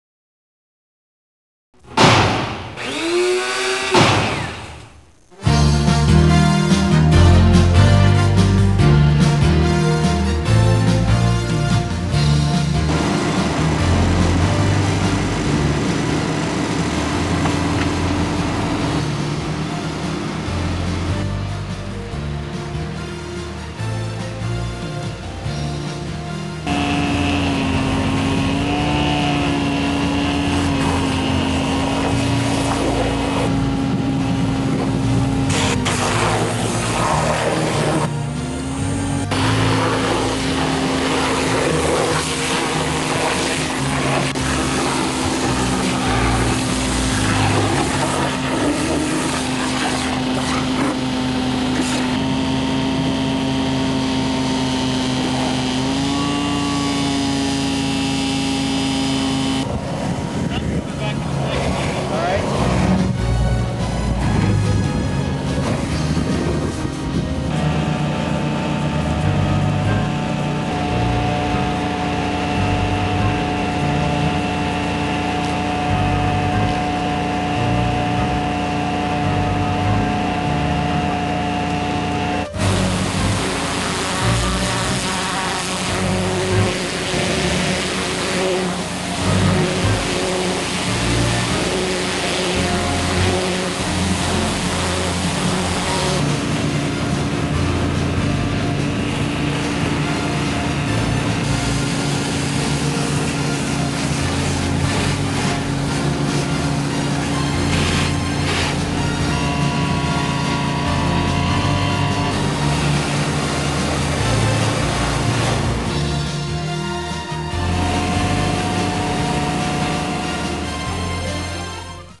Vac-Tron directional drilling clean up, potholing, and vacuum excavation
Vac-Tron's PMD series directional drilling, potholing and vacuum excavation demonstration. Vac-Tron's original signature collection safely locates underground utilities by vacuum, pulls line through conduits, installs short, horizontal bores, and removes drilling mud. Over 50 industrial vacuum uses wet and dry. The PMD 833 has now been replaced by the LP 855 which has a lower profile for easier maneuverability. Diesel and super diesel models. Hydraulically operated, full-open rear door. Waterproof, lockable control panel. All service points for engine, water pump and vacuum pump are accessible from curbside. Vac-Tron's product line can be utilized for pot holing, vacuum excavation, air excavation and hydro excavation to locate underground utilities and has dozens of municipal uses including utility line clean-out.